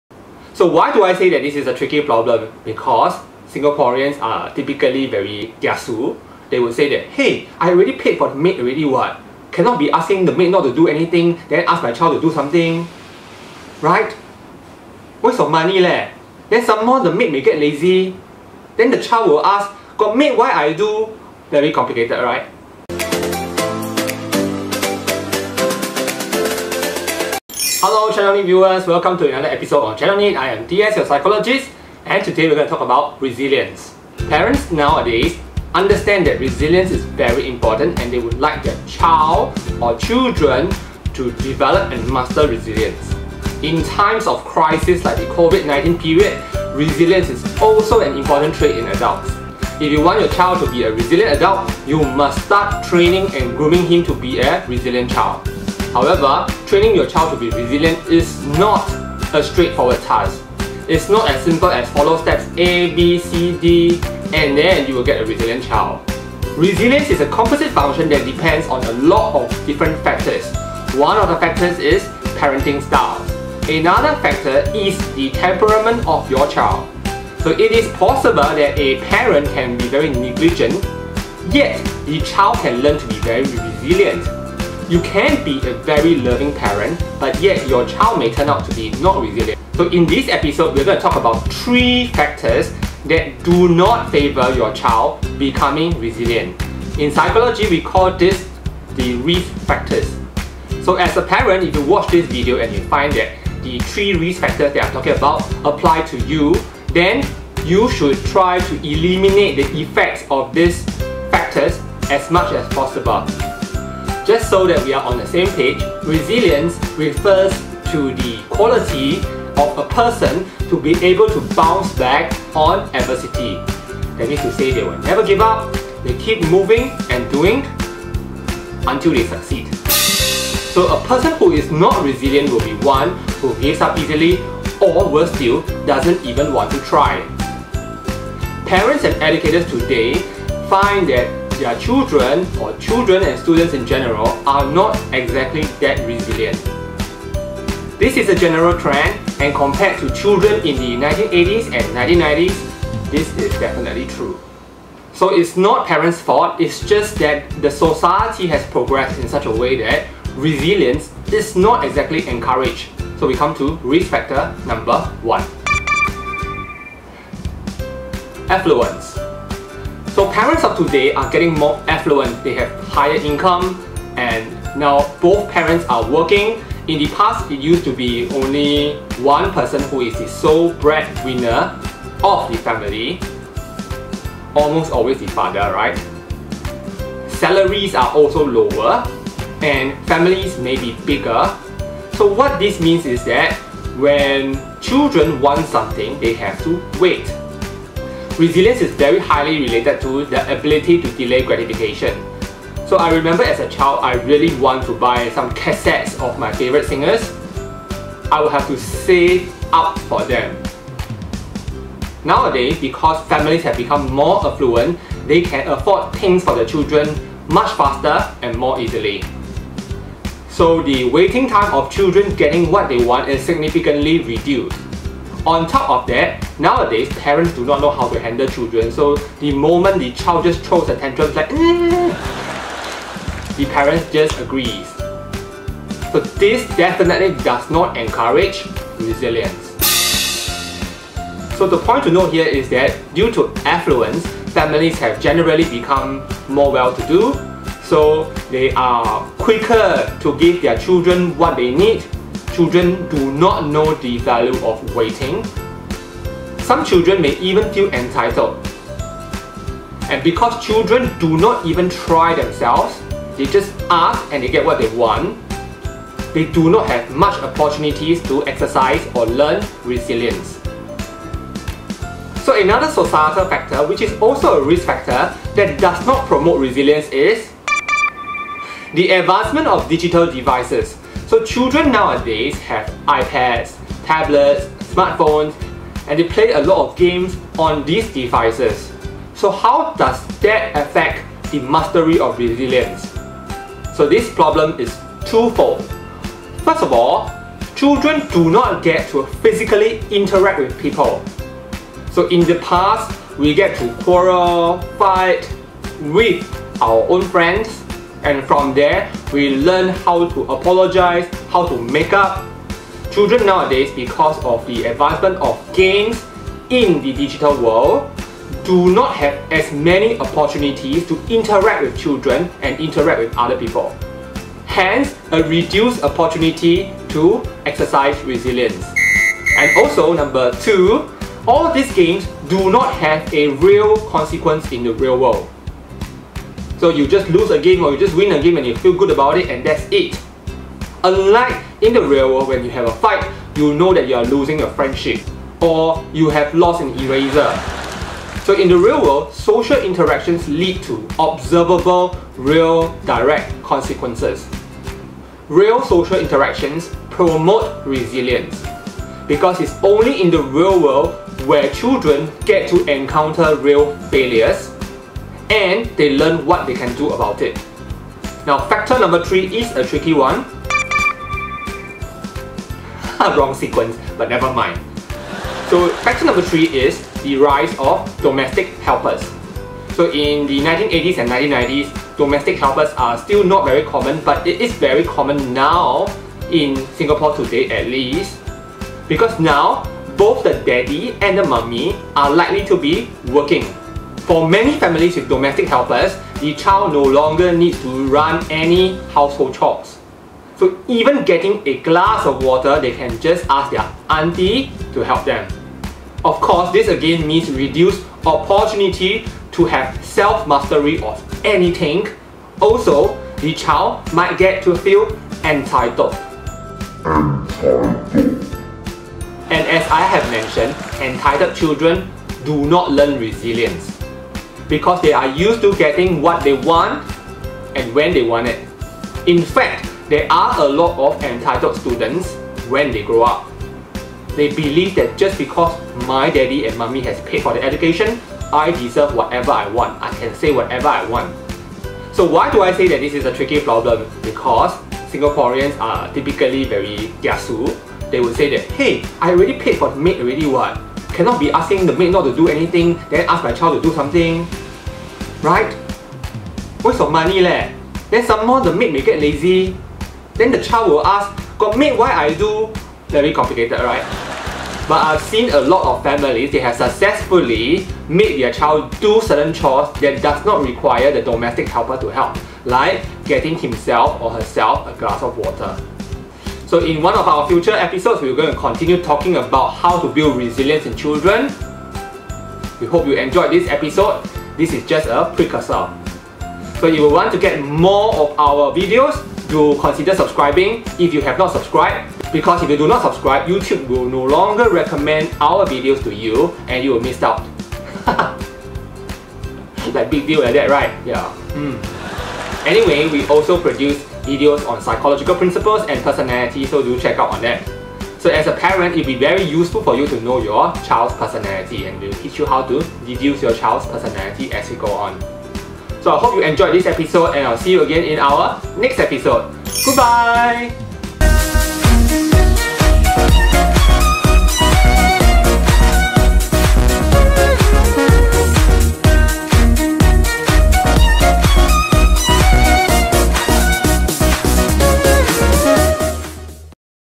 Ep 73A Building RESILIENCE in Children, Risk factors and why it is hard to build resilience
Resilience is an important success trait, almost all successful people are resilient. The COVID19 pandemic has once again attested how important it is for one to have resilience. Resilience or grit does not develop overnight and does not develop over a simple prescription. So how can we develop resilience? Before we come to that on another episode, let's talk about what the risk factors are: these factors generally do not encourage the training, grooming and building of resilience. So parents who watch this can think about how they can counter the effects of these risk factors pervading our times now. With proper management, their effects can be reduced (or even eliminated!)